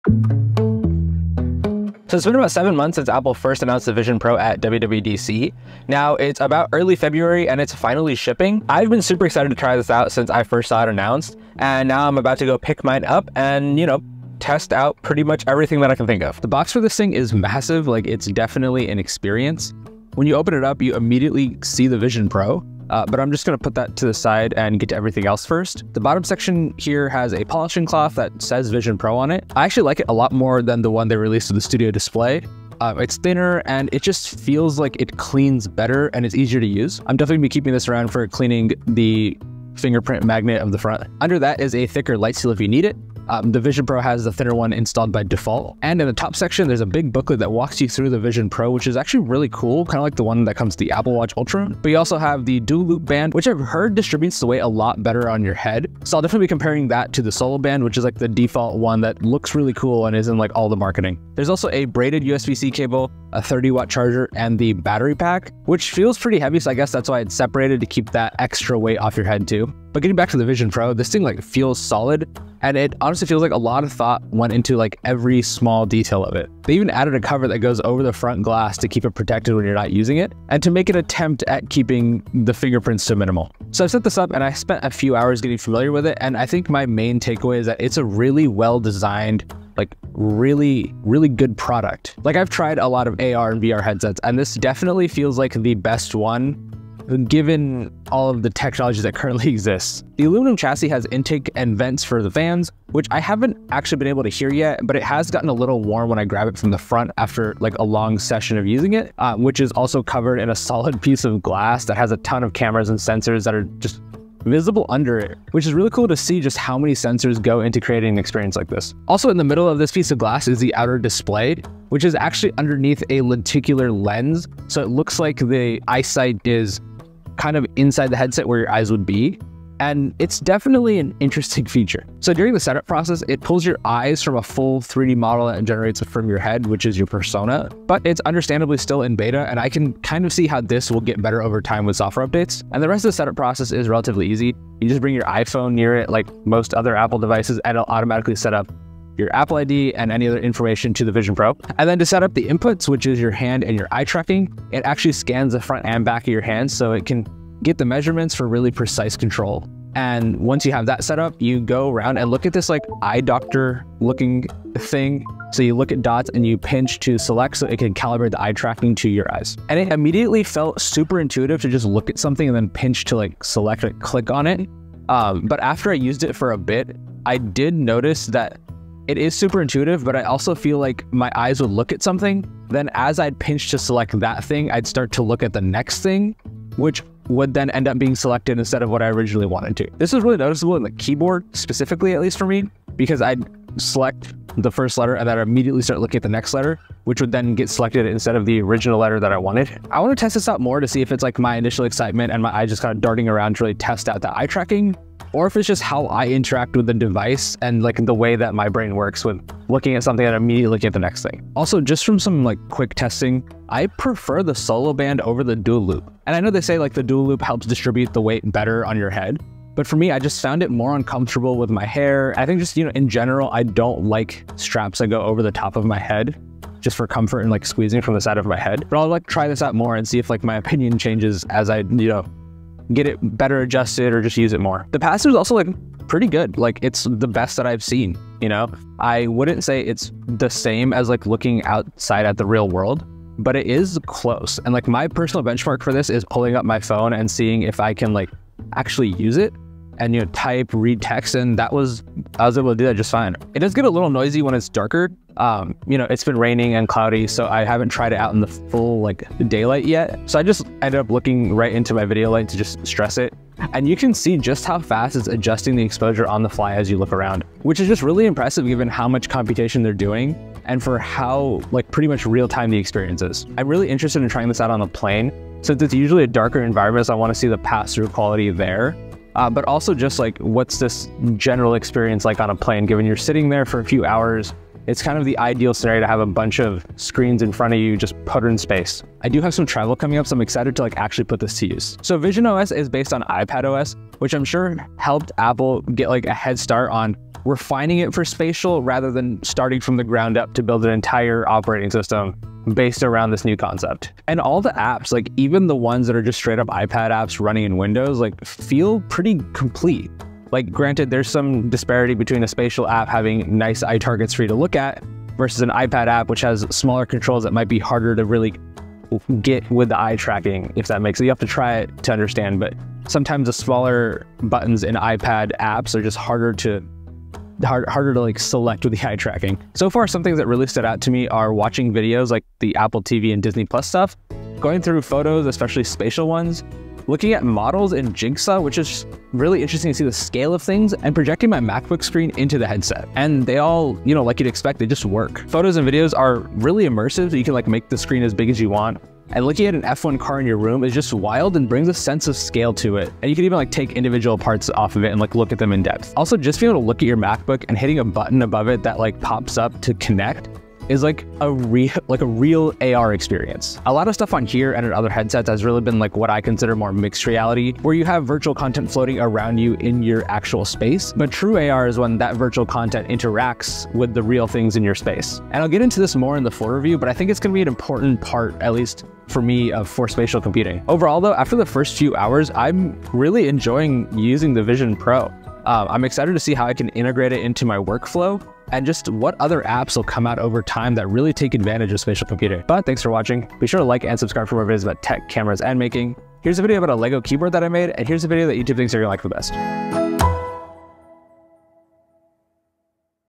Apple Vision Pro: First Impressions!
Apple’s Vision Pro finally shipped and it’s definitely one of the coolest devices this year!